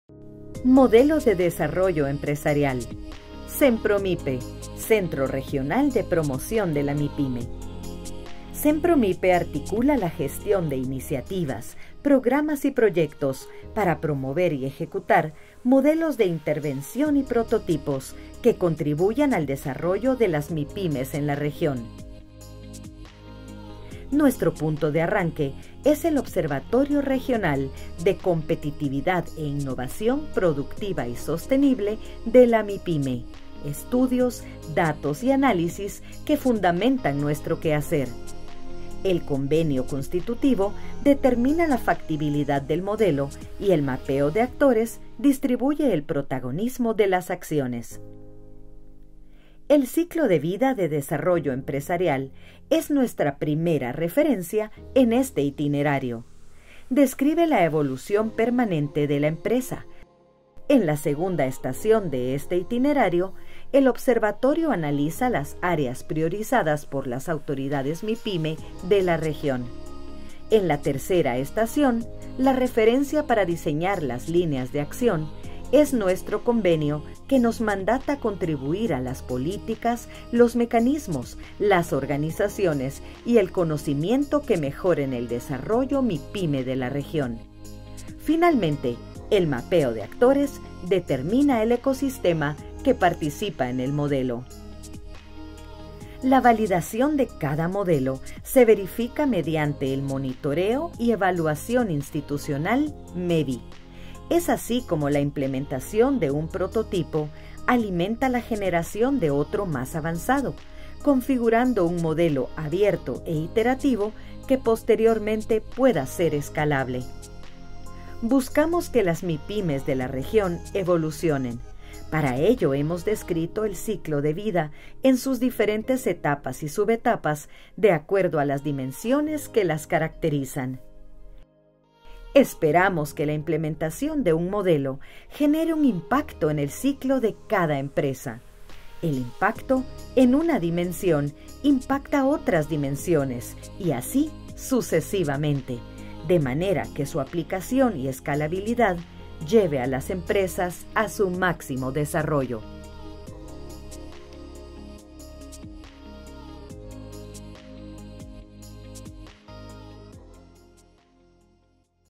Modelo de Desarrollo Empresarial
Cenpromype articula la gestión de iniciativas, programas y proyectos para promover y ejecutar modelos de intervención y prototipos que contribuyan al desarrollo de las mipymes en la región.

La implementación de cada prototipo genera un impacto integral en las empresas, su aplicación y escalabilidad permiten el desarrollo máximo de una empresa.